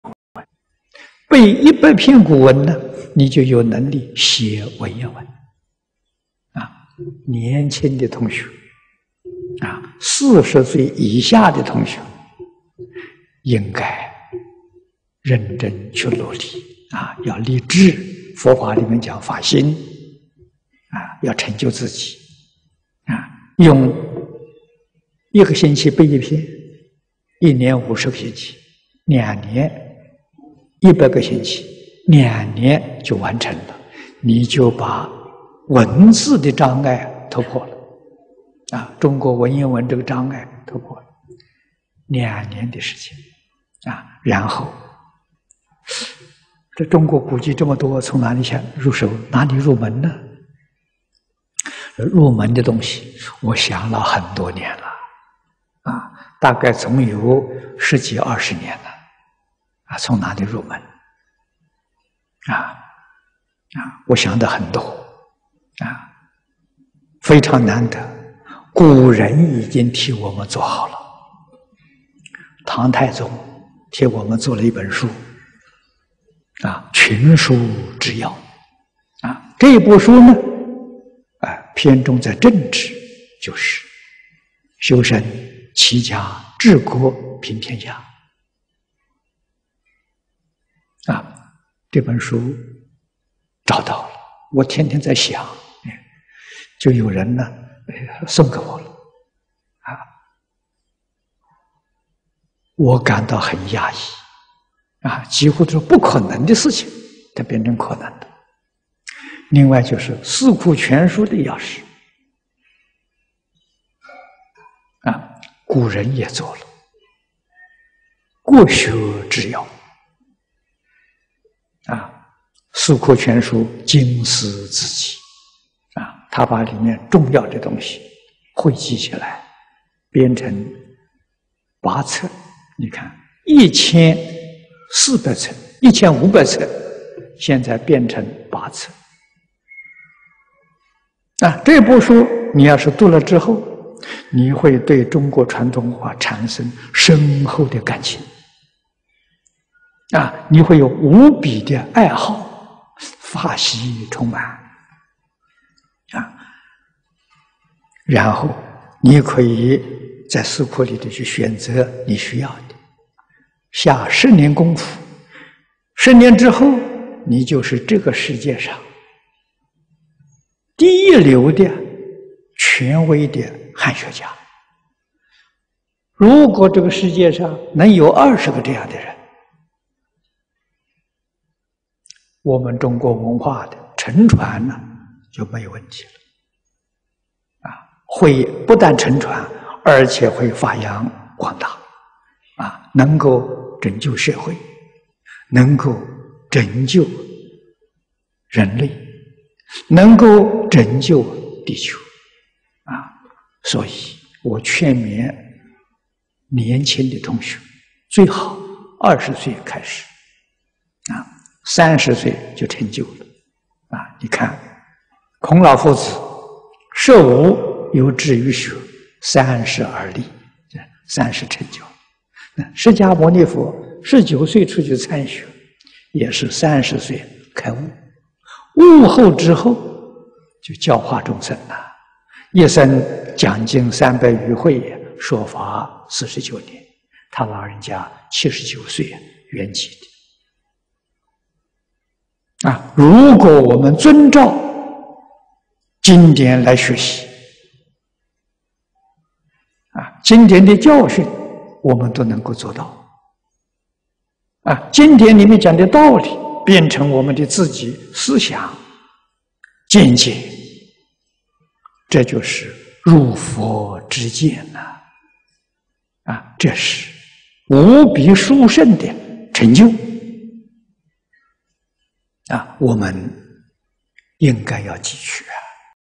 淨空老法師:繼承傳統文化、大法責任應當擺在第一位 6/8
~2011年新加坡弘法大會—淨空老法師開示
~新加坡博覽中心第一大廳

影片字幕：
背一百篇古文，你就有能力寫文言文。年輕的同學，四十歲以下的同學，應該認真去努力，要立志，佛法裡面講發心，要成就自己。用一個星期背一篇，一年五十個星期，兩年一百個星期，兩年就完成了，你就把文字的障礙突破了，中國文言文這個障礙突破了，兩年的時間。然後，中國古籍這麼多，從哪裡入手、哪裡入門？入門的東西我想了很多年，大概總有十幾二十年，從哪裡入門，我想得很多。非常難得，古人已經替我們做好了，唐太宗替我們做了一本書《群書治要》。這部書偏重在政治，就是修身、齊家、治國、平天下。這本書找到了，我天天在想，就有人送給我了，我感到很訝異，幾乎是不可能的事情，變成可能了。另外就是《四庫全書》的鑰匙，古人也做了，《國學治要》。《四庫全書》經史子集，它把裡面重要的東西會集起來編成八冊，你看，一千五百冊現在變成八冊。這部書你要是讀了之後，你會對中國傳統文化產生深厚的感情，你會有無比的愛好，法喜充滿。然後你可以在《四庫》裡頭去選擇你需要的，下十年功夫，十年之後你就是這個世界上第一流的、權威的漢學家。如果這個世界上能有二十個這樣的人，我們中國文化的承傳就沒有問題了；不但承傳，而且會發揚光大，能夠拯救社會，能夠拯救人類，能夠拯救地球。

        所以我勸勉年輕的同學，最好二十歲開始，三十歲就成就了。你看孔老夫子「十五有志於學，三十而立」，三十成就。釋迦牟尼佛十九歲出去參學，也是三十歲開悟，悟了之後就教化眾生，一生講經三百餘會，說法四十九年，他老人家七十九歲圓寂的。如果我們遵照經典來學習，經典的教訓我們都能夠做到，經典裡面講的道理變成我們自己的思想、見解，這就是入佛知見，這是無比殊勝的成就，我們應該要汲取。